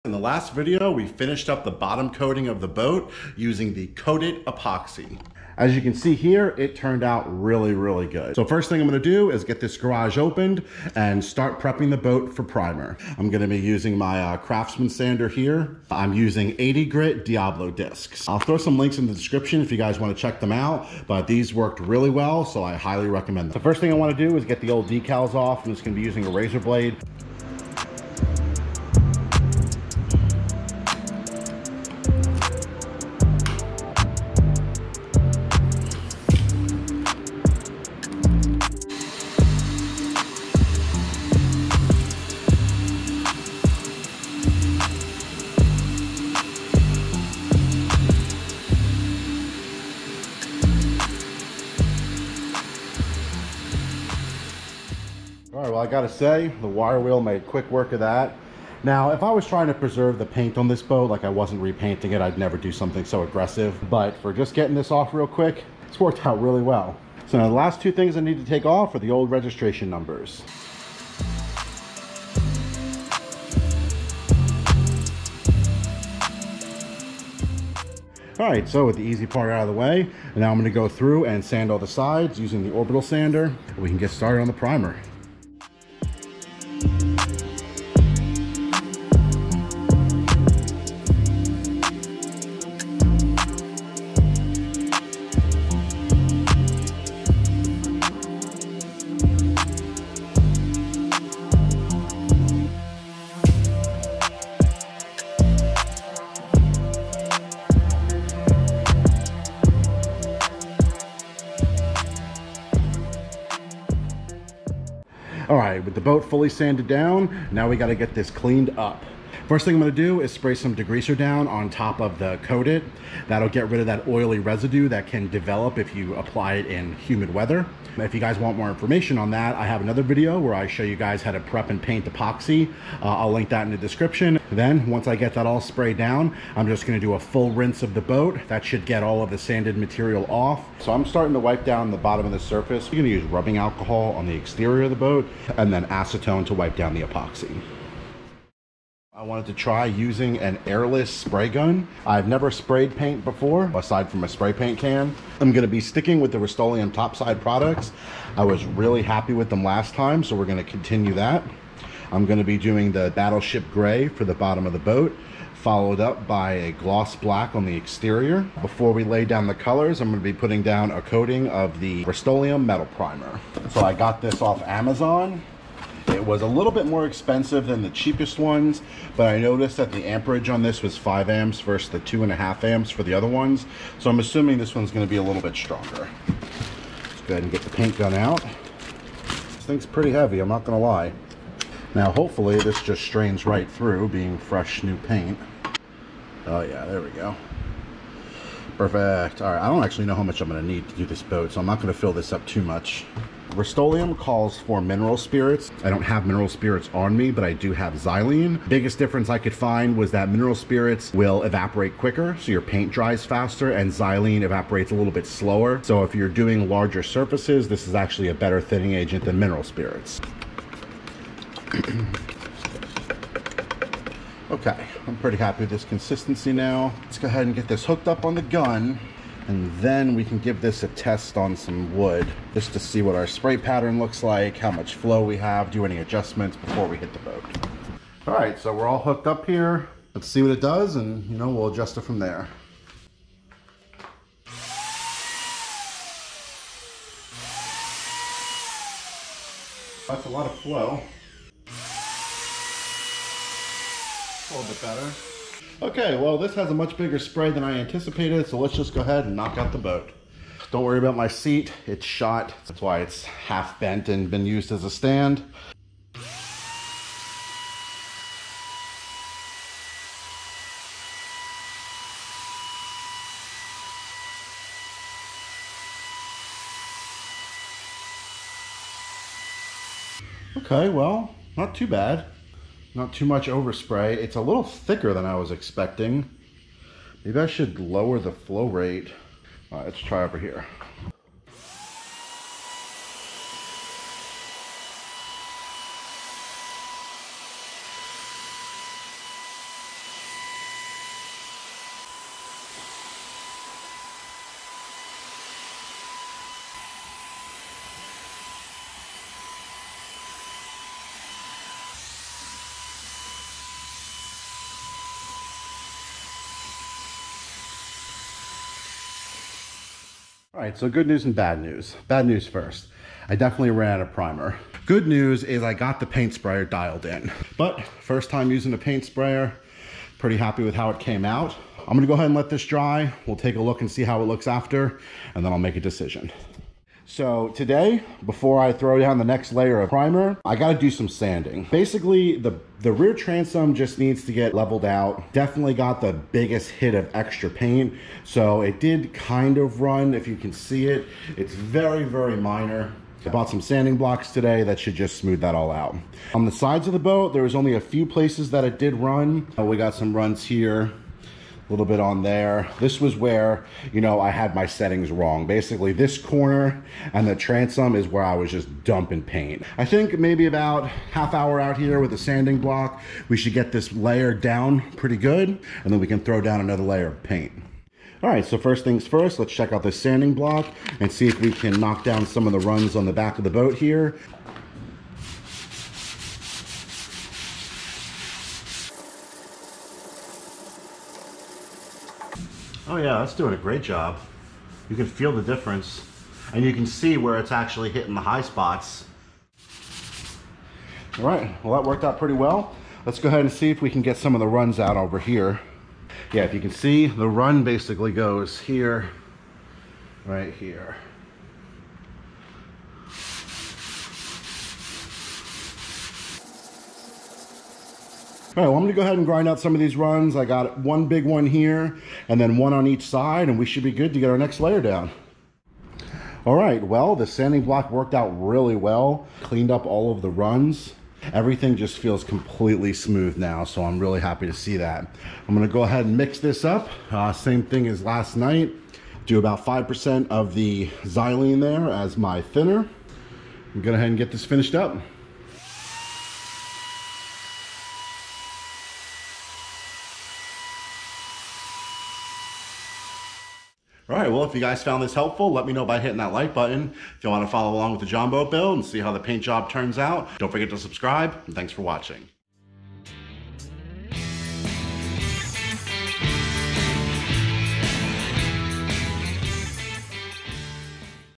RUSTOLEUM MARINE METAL PRIMER - PREP & PAINT - Jon Boat to Bass Boat Conversion Topside
- The Jon Boat is finally ready for painting! In this video I go over the prep & primer process I used for the first stage of the Jon Boat paint job.

–––Product Links*
Electric Paint Sprayer
Foam Roller Kit
Rust-Oleum Marine Topside Paint
Rust-Oleum Marine Metal Primer
Diablo Sanding Discs
Wire Brush Wheel Set